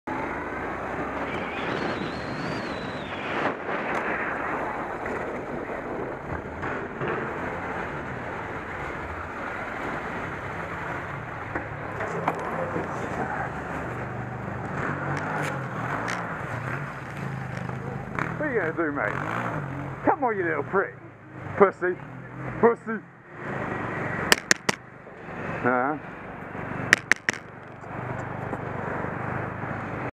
Motorcycle Theft Attempt London
Today was exciting 👍.

Two morons attempted to steal my motorbike - however as they are basically offspring from sand rats, I didn't have any problem dealing with their pathetic threat.